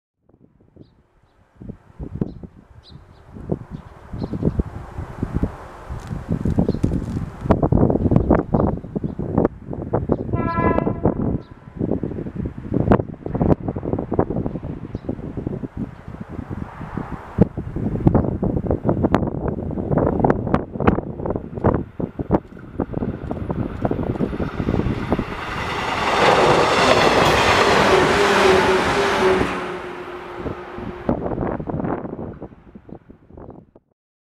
【警笛あり!!】115系300番台D-27編成
東岡山駅を出て20分ほど歩いたところにある山陽本線の警笛ポイントにて撮影しました。